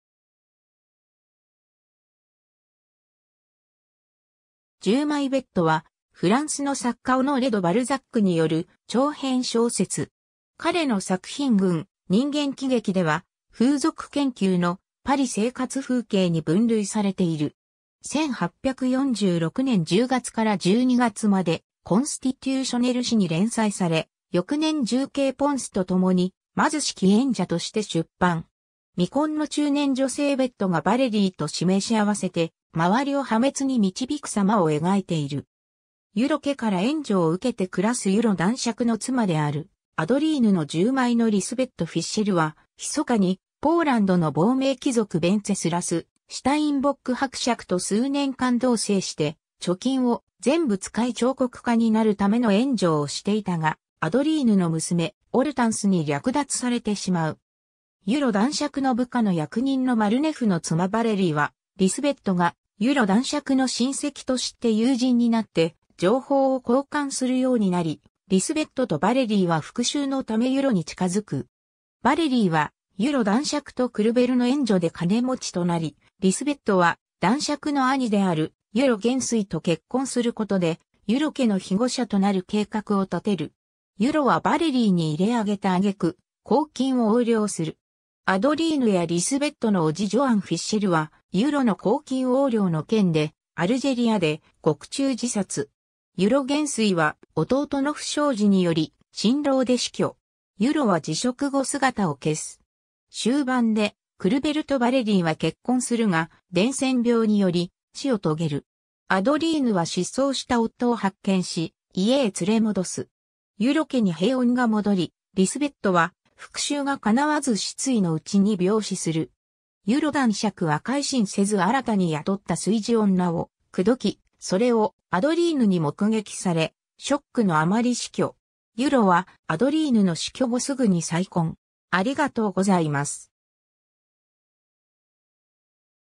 従妹ベット
オノレ・ド・バルザックの小説
『従妹ベット』（いとこベット、仏：La Cousine Bette）はフランスの作家オノレ・ド・バルザックによる長編小説。
 彼の作品群『人間喜劇』では「風俗研究」の「パリ生活風景」に分類されている。
 1846年10月～12月までコンスティテューショネル紙(fr:Le Constitutionnel)に連載され、翌年従兄ポンスと共に「貧しき縁者」として出版。
 未婚の中年女性ベットがヴァレリーと示し合せて、周りを破滅に導くさまを描いている。
 ユロ家から援助を受けて暮らすユロ男爵の妻であるアドリーヌの従妹のリスベット・フィッシェルは、密かにポーランドの亡命貴族ヴェンツェスラス・シタインボック伯爵と数年間同棲して貯金を全部使い彫刻家になる為の援助をしていたが、アドリーヌの娘・オルタンスに略奪されてしまう。
 ユロ男爵の部下の役人のマルネフの妻ヴァレリーは、リスベットがユロ男爵の親戚と知って友人になって情報を交換するようになり、リスベットとヴァレリーは復讐の為ユロに近付く。
 ヴァレリーはユロ男爵とクルヴェルの援助で金持ちとなり、リスベットは男爵の兄であるユロ元帥と結婚する事でユロ家の庇護者となる計画を立てる。
ユロはヴァレリーに入れ揚げた挙句、公金を横領する。
アドリーヌやリスベットの叔父ジョアン・フィッシェルはユロの公金横領の件でアルジェリアで獄中自殺。
ユロ元帥は弟の不祥事により心労で死去。
ユロは辞職後姿を消す。
 終盤でクルヴェルとヴァレリーは結婚するが、伝染病により死を遂げる。
アドリーヌは失踪した夫を発見し、家へ連れ戻す。
ユロ家に平穏が戻り、リスベットは復讐が叶わず失意の内に病死する。
 ユロ男爵は改心せず新たに雇った炊事女を口説き、それをアドリーヌに目撃されショックの余り死去。
ユロはアドリーヌの死去後すぐに再婚。